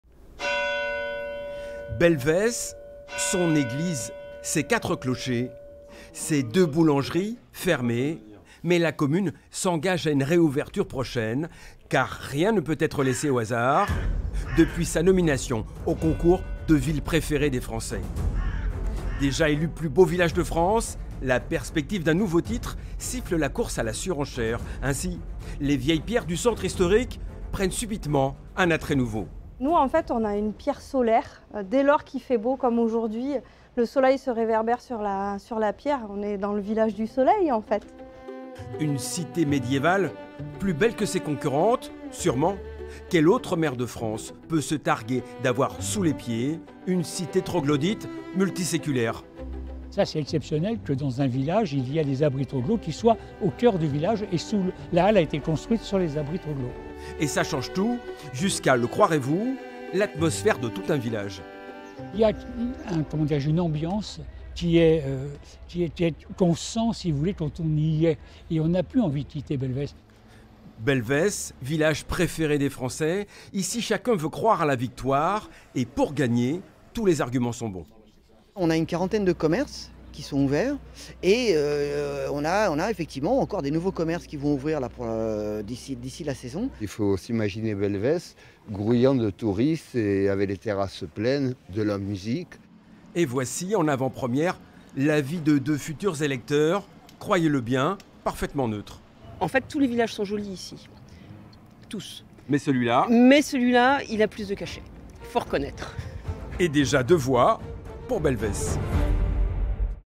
Belvès, candidat, village préféré des français
Le village de Dordogne, déjà plus beau village de France, nominé pour le concours de village préféré des français.